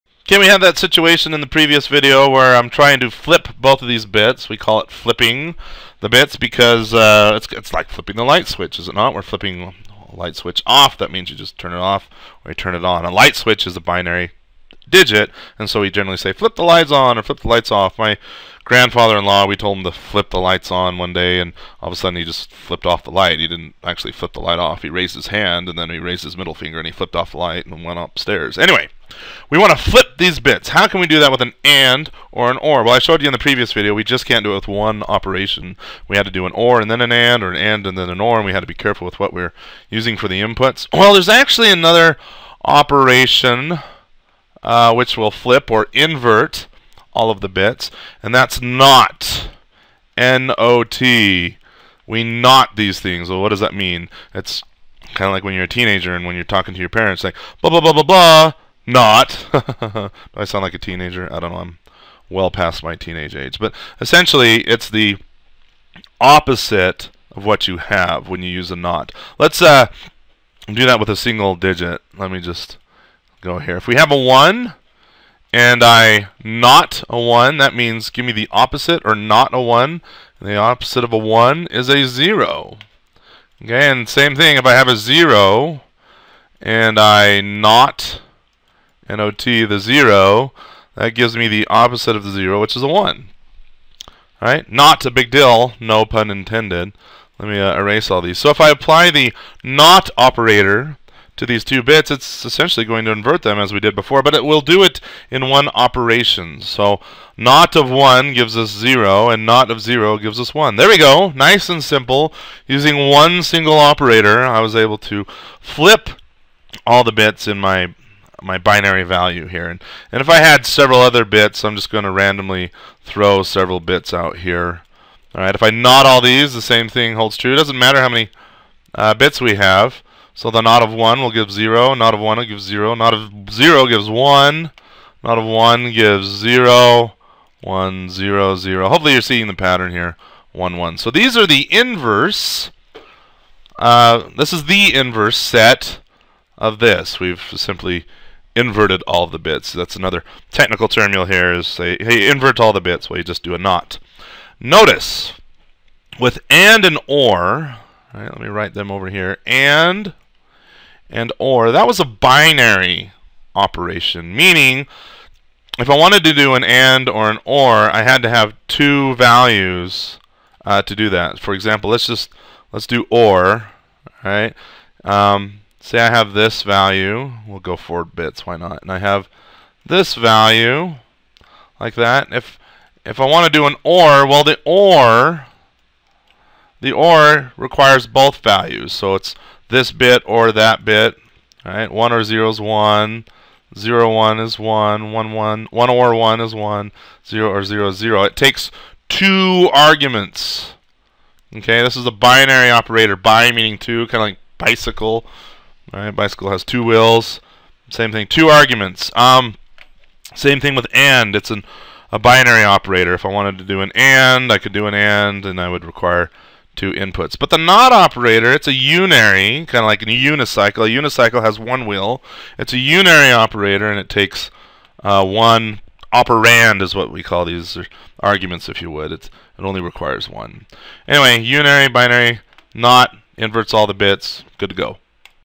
Bit Fields- Not Operator
Jamie King showing the NOT operation to invert (flip) all the bits.